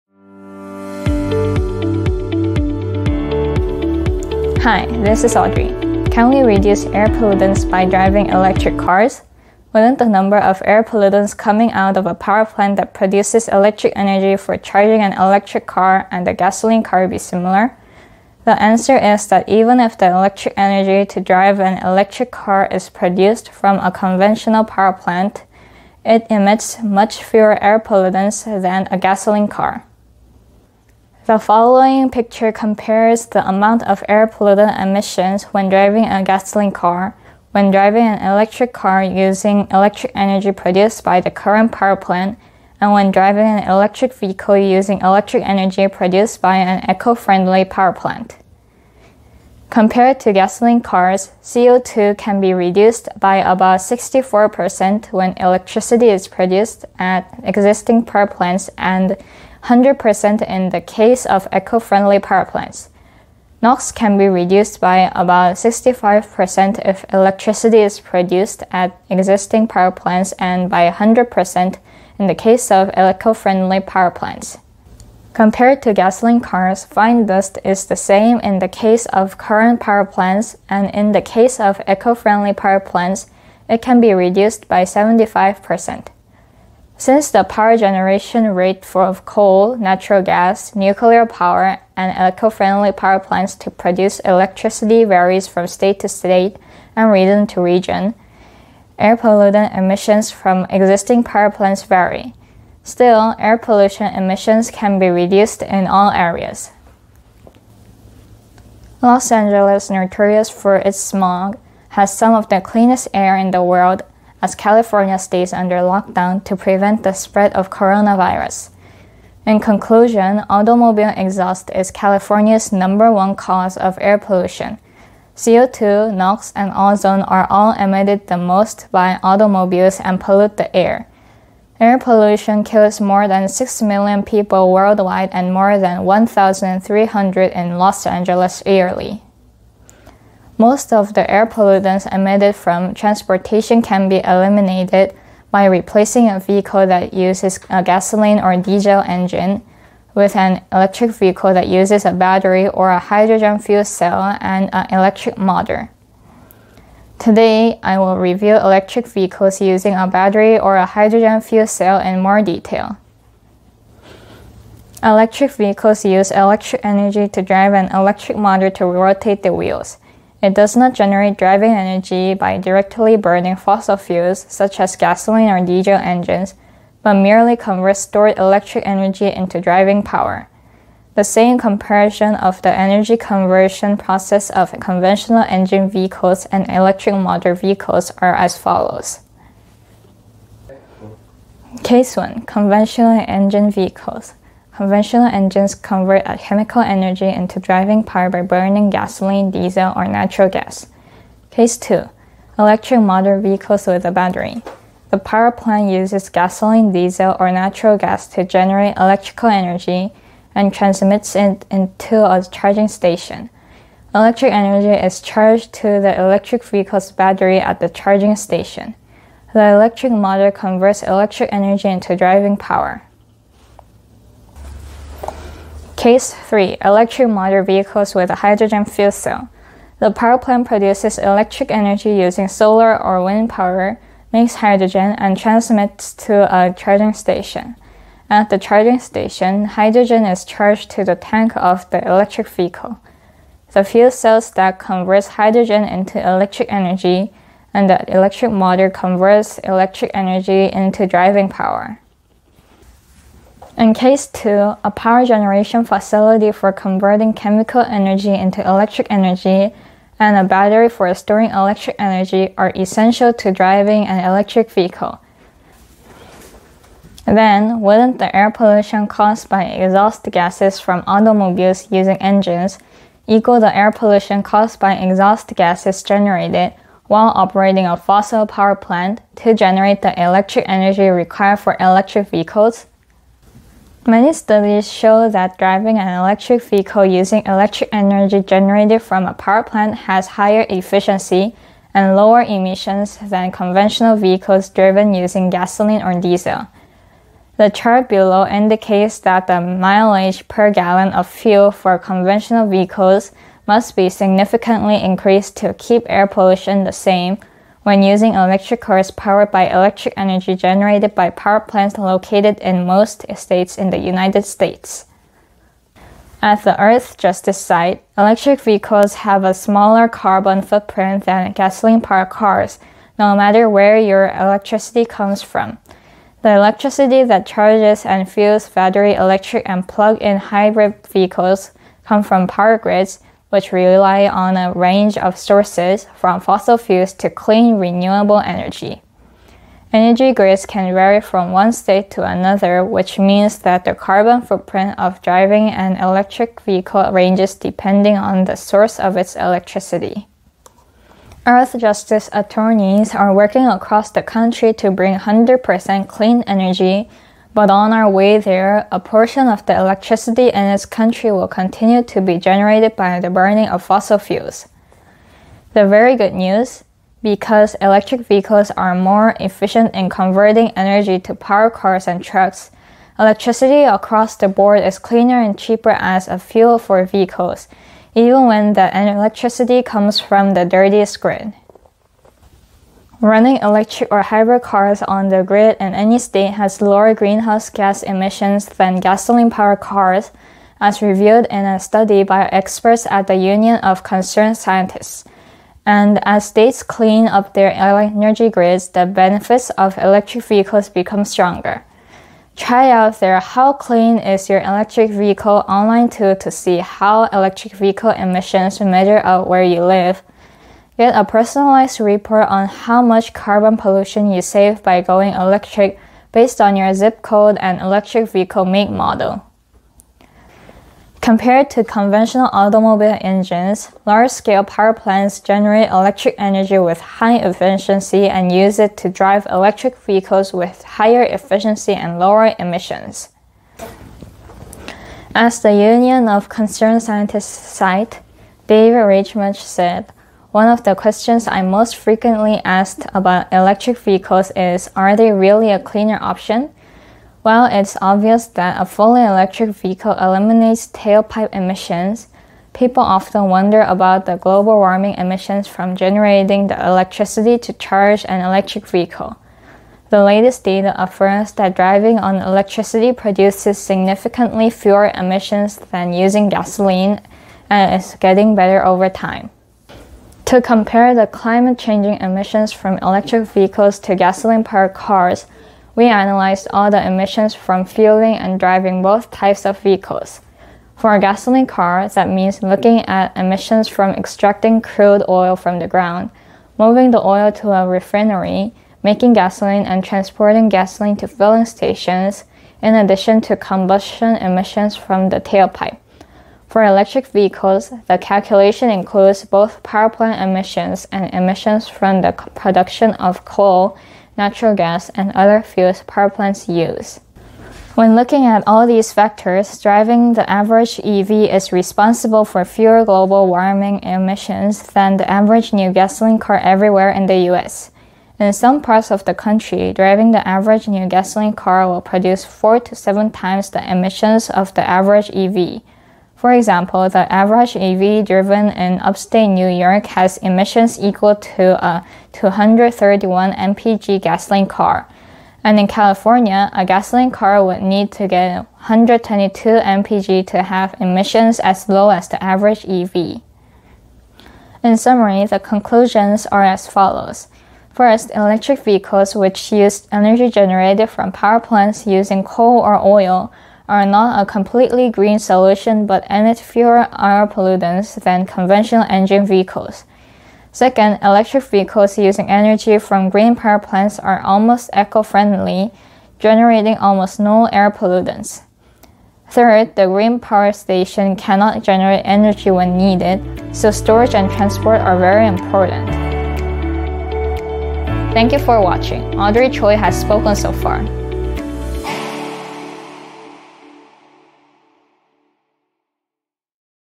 Is electric car better for the air, enviroment than the gas car ? Audrey Choi
Are electric cars better for the air the gasoline car ?

This is Audrey Choi
Can we reduce air pollutants by driving electric cars?
Wouldn't the number of air pollutants coming out of a power plant that produces electric energy for charging an electric car and a gasoline car be similar?
The answer is that even if the electric energy to drive an electric car is produced from a conventional power plant, it emits much fewer air pollutants than a gasoline car.

In summary, the conclusions are as follows.
First, electric vehicles, which use energy generated from power plants using coal or oil, are not a completely green solution but emit fewer air pollutants than conventional engine vehicles.
Second, electric vehicles using energy from green power plants are almost eco-friendly, generating almost no air pollutants.
Third, the green power station cannot generate energy when needed, so storage and transport are very important.